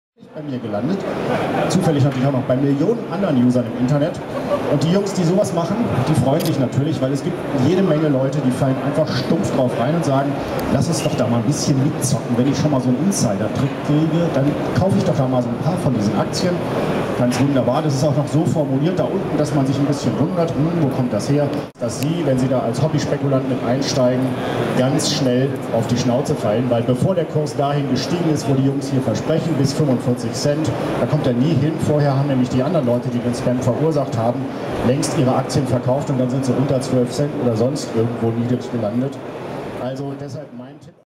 Georg Schnurer - CeBIT 2008
Chefredakteur der c't Georg Schnurer bei einem Vortrag auf der CeBIT 2008